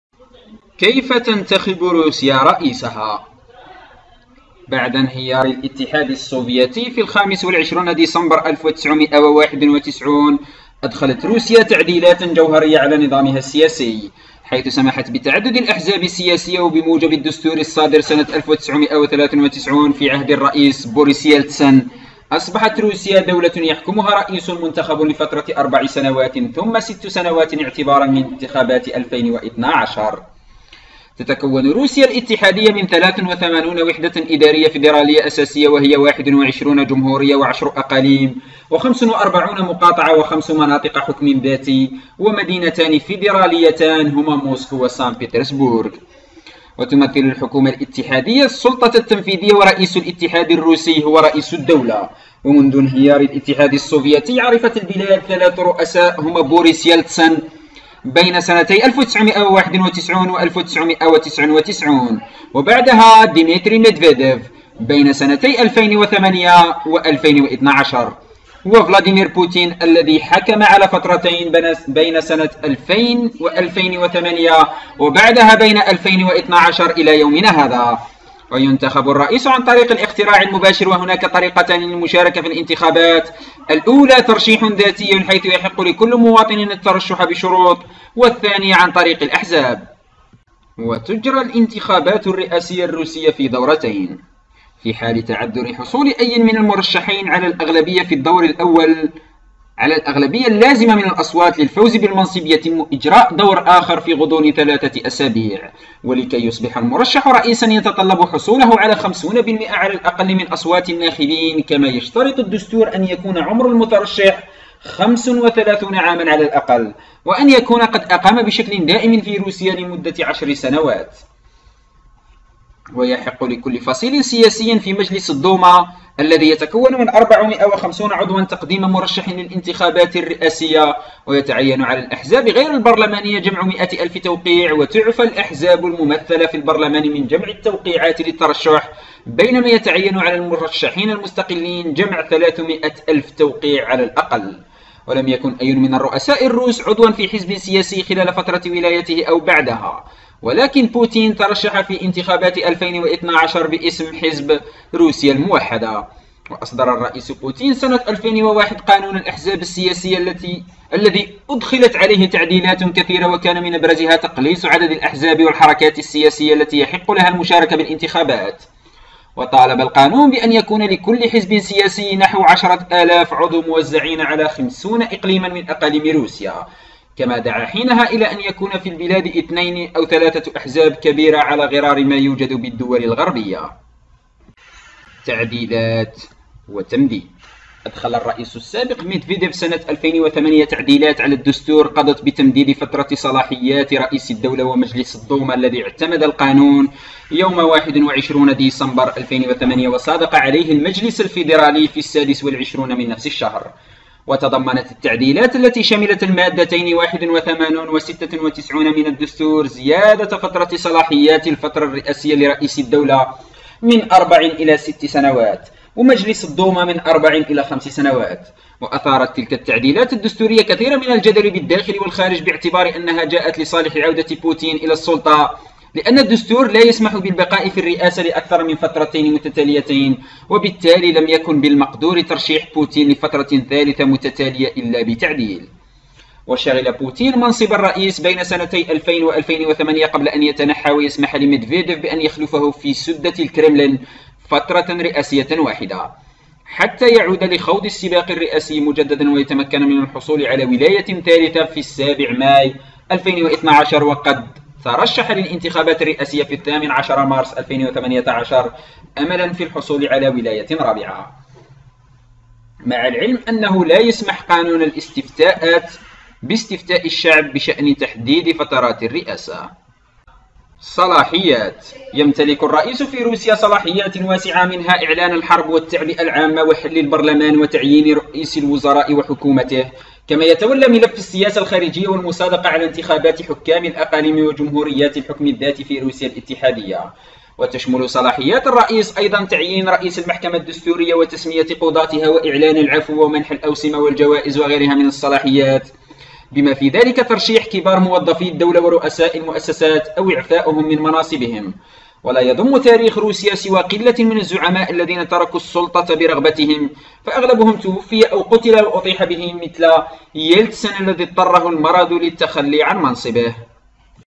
كيف تنتخب روسيا رئيسها ؟
بعد انهيار الاتحاد السوفياتي في 25 ديسمبر/كانون الأول 1991 أدخلت روسيا تعديلات جوهرية على نظامها السياسي، حيث سمحت بتعدد الأحزاب السياسية، وبموجب الدستور الصادر عام 1993 في عهد الرئيس بوريس يلتسين، أصبحت روسيا دولة يحكمها رئيس منتخب لفترة أربع سنوات ثم ست سنوات اعتبارا من انتخابات 2012.

After the collapse of the Soviet Union on December 25, 1991, Russia made fundamental amendments to its political system, allowing the plurality of political parties. 2012 elections.